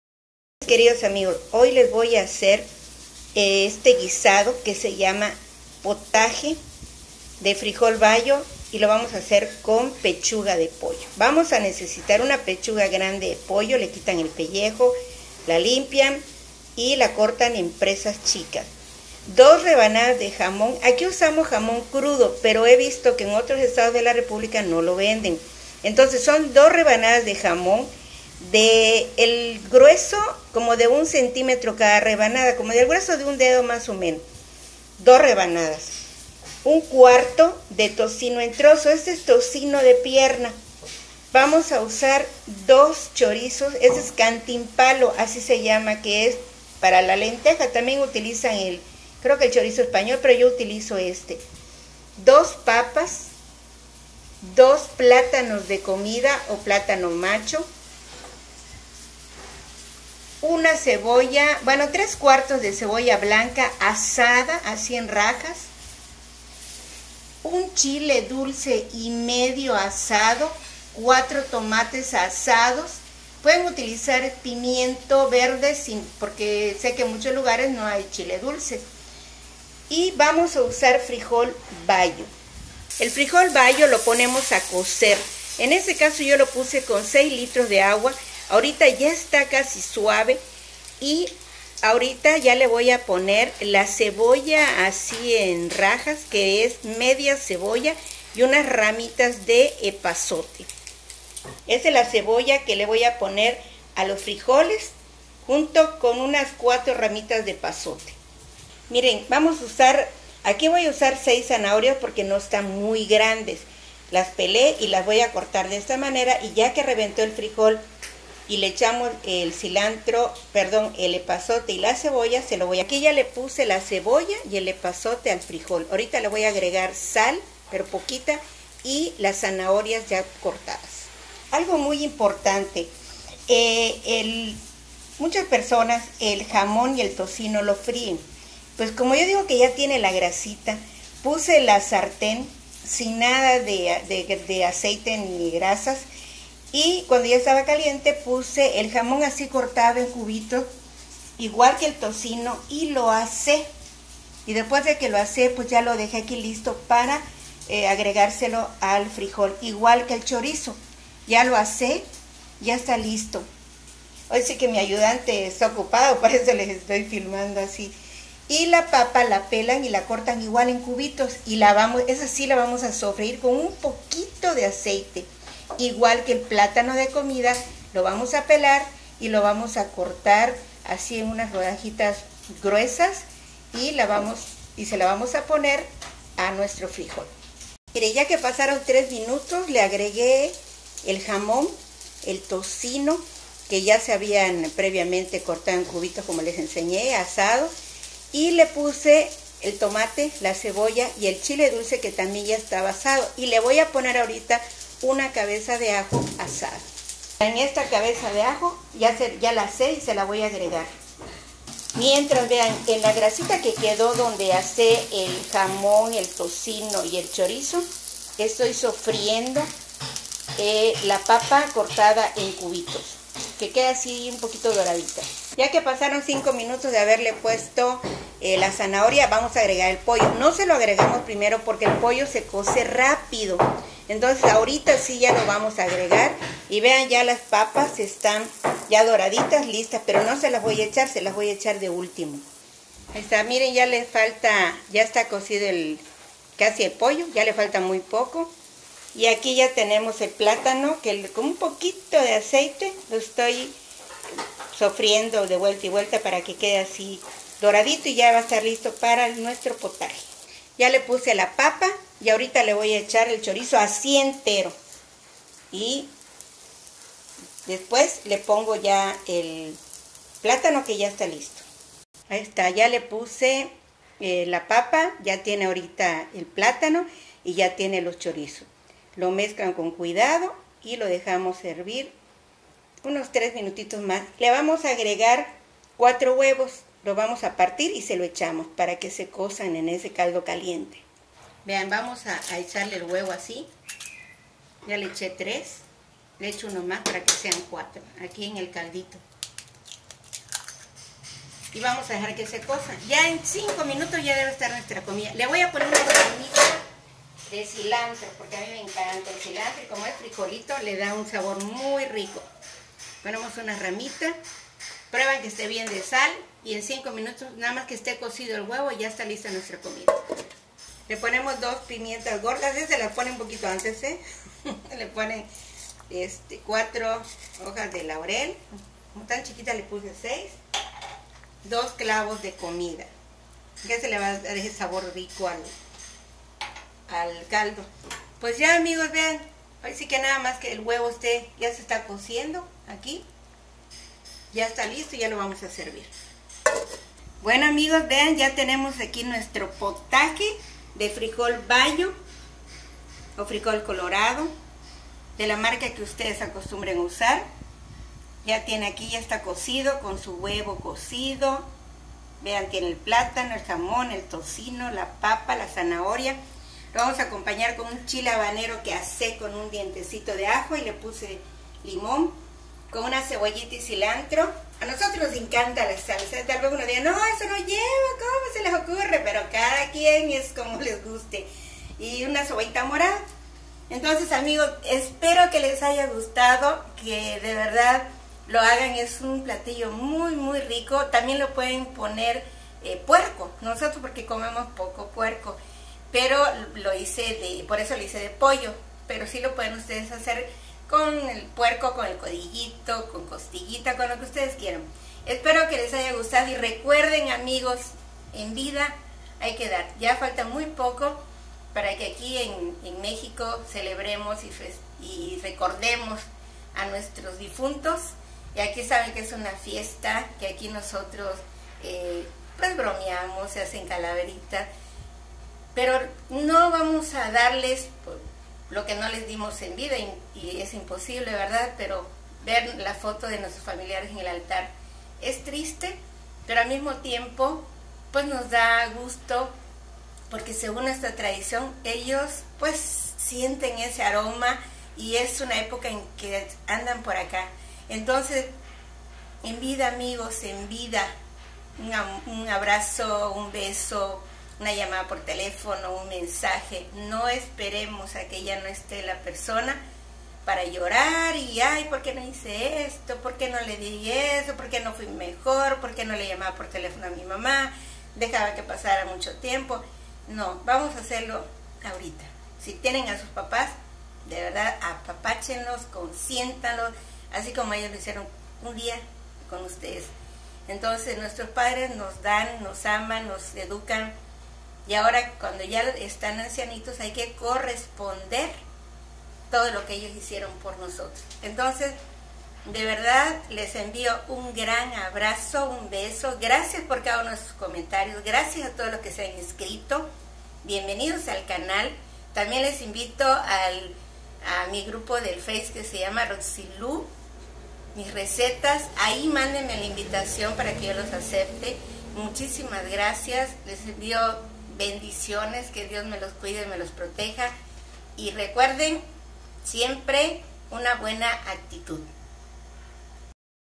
Potaje de Frijol Bayo con Pollo
Ingredientes

Rinde para 8 u 10 personas

1 Pechuga de pollo grande cortada en presas chicas
1 Kg. De Frijol Bayo o Colorado
2 Rebanadas de jamón crudo ( O el jamón que acostumbren a comer) un centímetro de ancho cada una, lo cortan en cubitos y los asan
1/4 De Tocino cortado en cubitos y asado
3/4 De Cebolla blanca cortada en rajas la asan
1/2 Cebolla blanca cortada en rajas para el frijol
4 Tomates Guajillos cortados en rajas sin semillas
1 1/2 Chiles dulces cortado en rajas sin semillas y asado ( O Pimiento morrón verde)
1 Cabeza de ajo asada
6 Zanahorias chicas peladas y cortadas en rodajas
2 Papas peladas y cortadas en cubitos sofritos
2 Chorizos Cantimpalos asados
4 Huevos (Para echarle al caldo)
4 Hojas de Laurel
2 Pimientas de Tabasco
2 Clavos de comida
1 Ramo de Epazote
Cilantro es al gusto